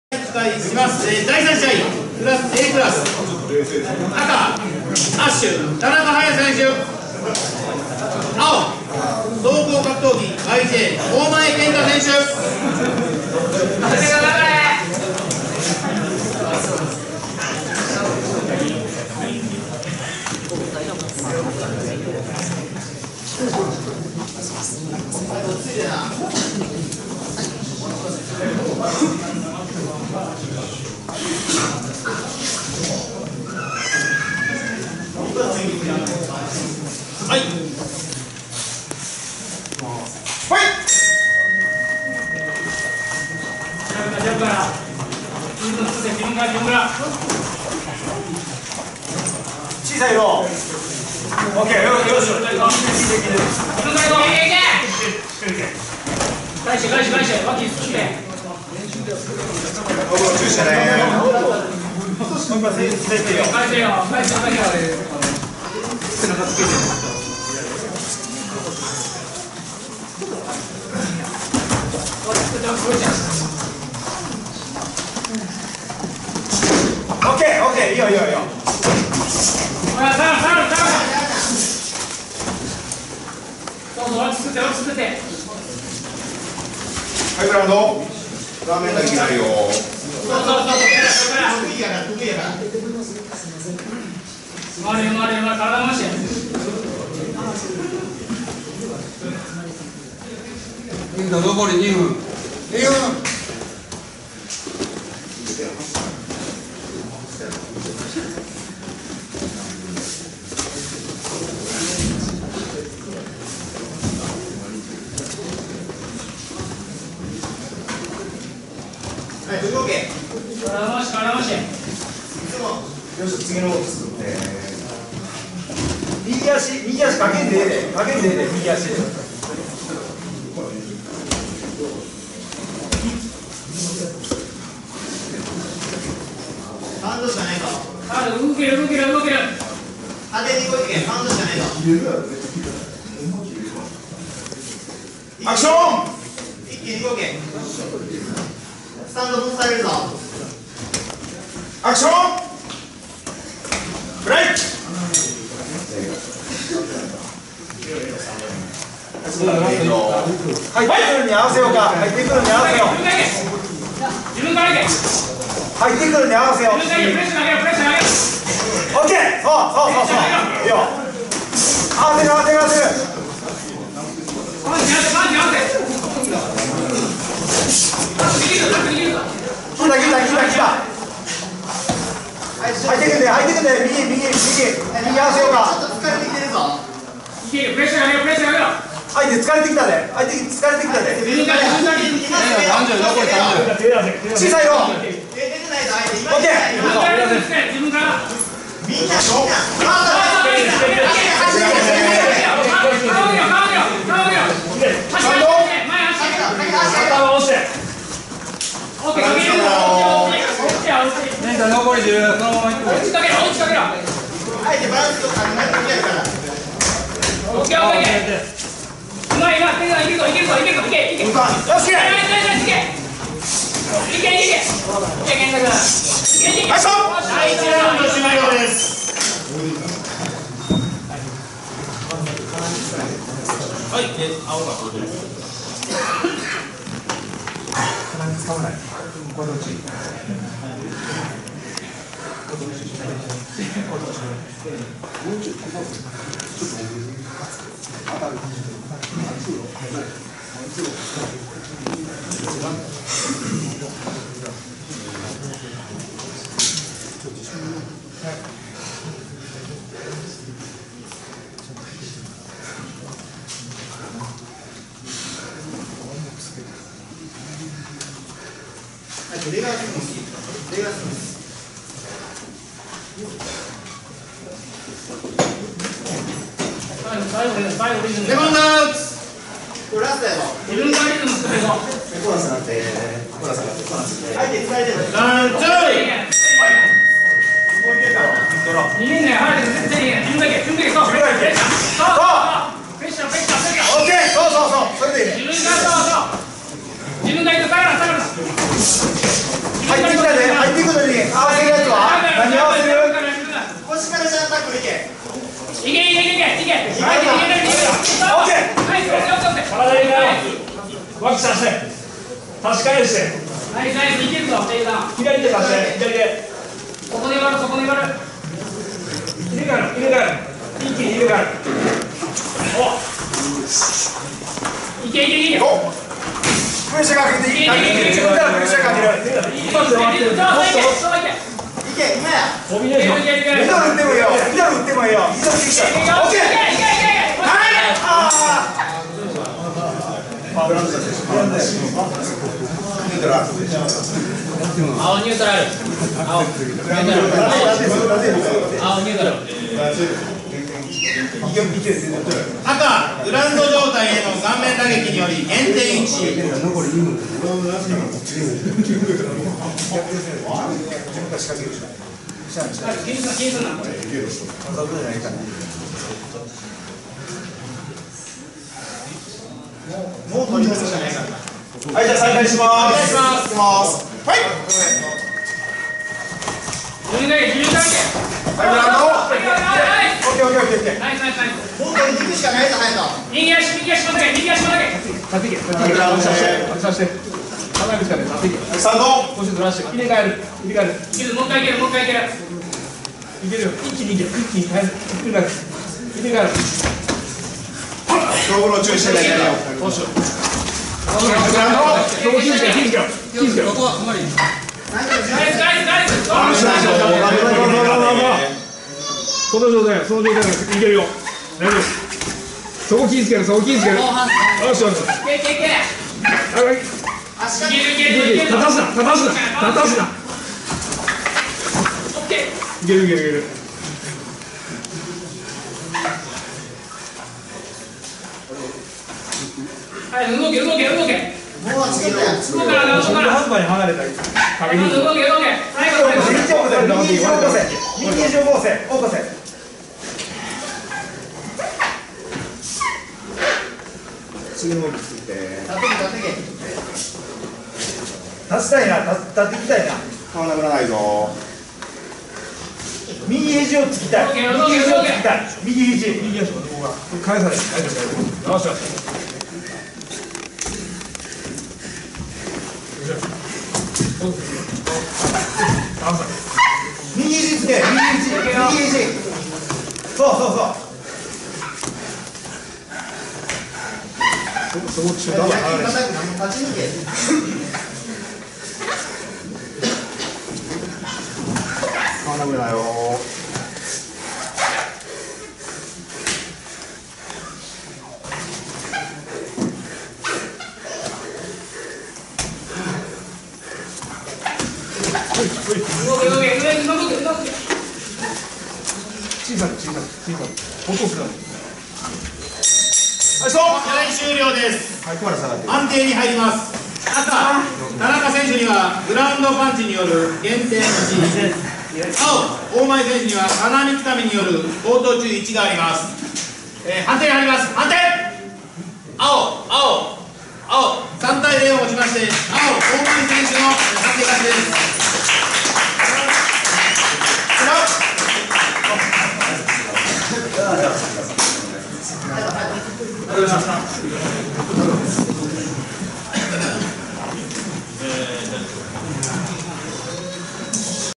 JMMAF大阪ケージファイト05③田中勇人vs大前健太
2018/03/11JMMAF大阪ケージファイト05＠パラエストラ大阪
第3試合　Aクラス61.2kg
✖️田中勇人（ASH)
vs
◯大前健太（総合格闘技YJ)